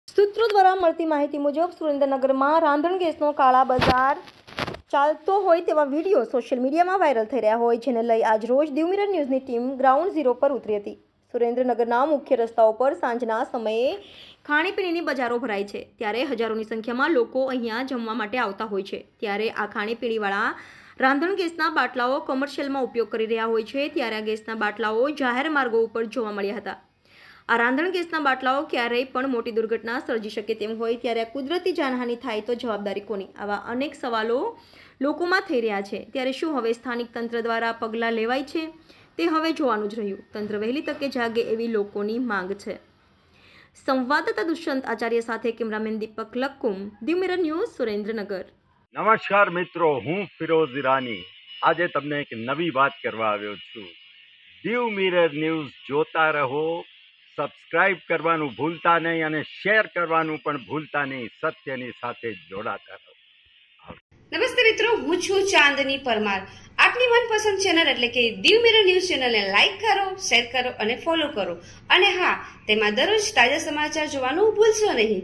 સુરેન્દ્રનગર માં રાંધણગેસ નો કાળાબઝાર સાલતો હોય તેવા વિડિયો સોષ્યલ મીડિયા માં || Surendranagar
2024 || Lok Sabha || Election || Congress || BJP || Narendra modi || Rahul gandhi || NDA || INDIA || Bharatiyajanataparty || Opinionpoll2024 || News || Live || Election ||